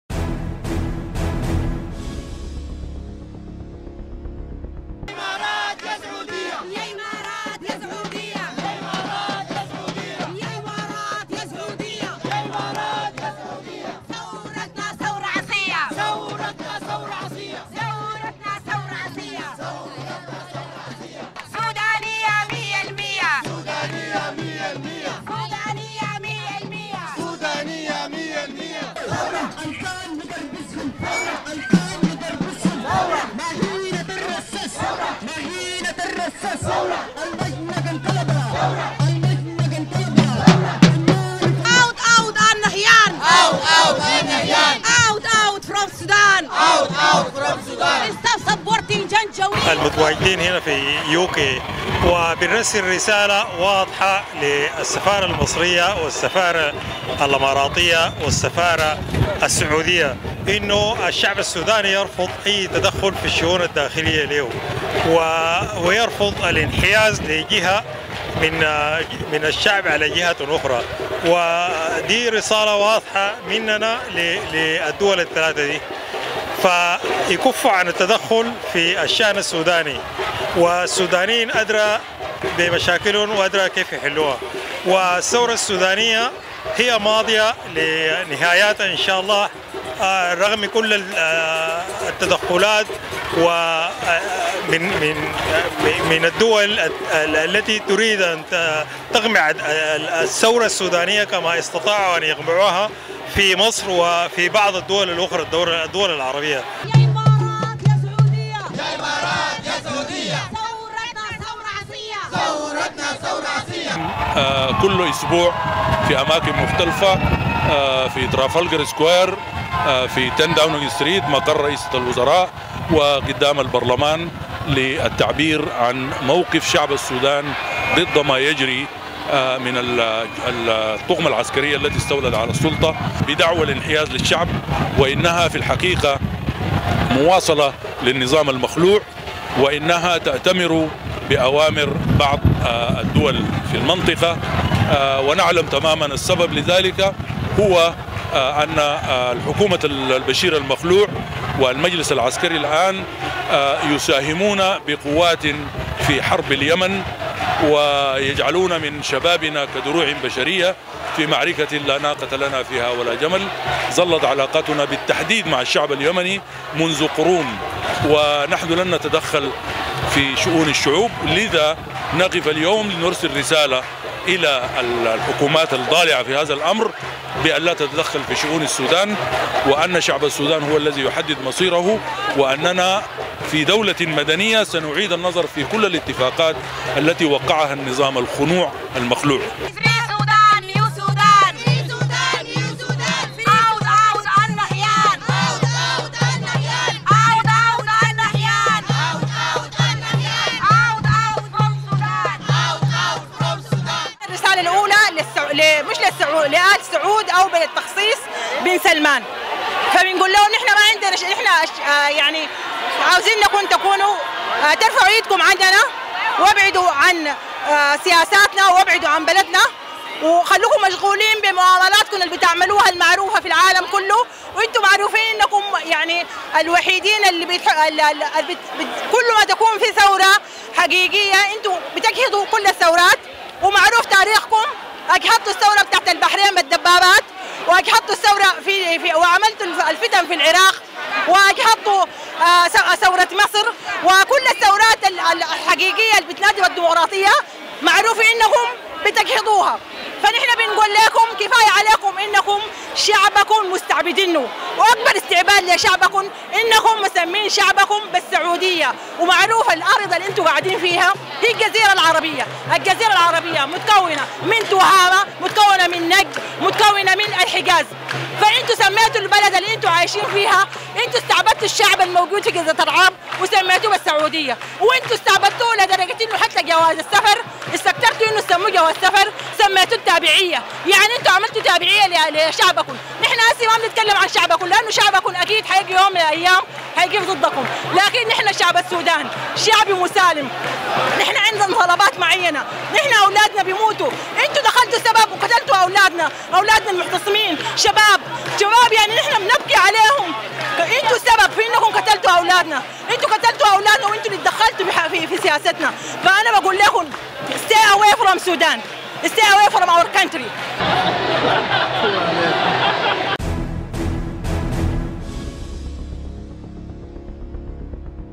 لندن: مسيرة صاخبة ضد تآمر السعودية والإمارات على الثورة السودانية
تظاهر مئات السودانيين وسط العاصمة البريطانية لندن يوم السبت الماضي 15 يونيو ضد دعم السعودية والإمارات للمجلس العسكري في السودان.
وسارت التظاهرة من سفارة الإمارات في لندن مرورا بحديقة “الهايد بارك” الشهيرة لتتوقف عند السفارة السعودية. وأطلق المتظاهرون شعارات لقوى الثورة المضادة من بينها “يا إمارات يا سعودية .. ثورتنا ثورة عصية”. واتهم المتظاهرون النظام السعودي والإماراتي بالتآمر على ثورة السودان، بدعم المجلس العسكري الذي قتل المتظاهرين السلميين، لمنع التغيير الحقيقي من خلال الثورة.
وقال رئيس المكتب الإجتماعي لرابطة القانونيين في المملكة المتحدة محمد أحمد، فاليكفو عن التدخل في الشأن السوداني، فالسودانيون أدرى بمشاكلهم وبطريقة حلها”. وأكد محمد أحمد أن “الثورة السودانية ماضية إلى نهاياتها رغم كل التدخلات التي تنوي قمعها على شاكلة قمعثورة البحرين ومصر”.
من جانبه قال القيادي في الحزب الشيوعي السوداني راشد سيد أحمد الشيخ “ إن المجلس العسكري هو مواصلة للنظام المخلوع، وهي تأتمر بأوامر بعض الدول الخليجية (الإمارات والسعودية) لأسباب معروفة”، وأضاف الشيخ إن حكومة البشير المخلوع والمجلس العسكري يساهمون بقوات في العدوات على اليمن، ويجعلون من شبابنا دروع بشرية في حرب ليس لنا فيها ناقة ولا جمل”. وأكد راشد الشيخ أن علاقتنا مع الشعب اليمني قوية منذ قرون، ونرفض التدخل في شؤون الشعوب، ولهذا نرسل من خلال المظاهرة رسالة للدول الضالعة في هذا الأمر بأن لا يتدخلوا في شؤون السودان”.

يذكر أن الجالية السودانية عكفت على تنظيم الوقفات الإجتجاجية بشكل أسبوعي في أماكن مختلفة من العاصمة البريطانية بشكل شبه أسبوعي منذ تفجر الثورة في السودان أبريل الماضي.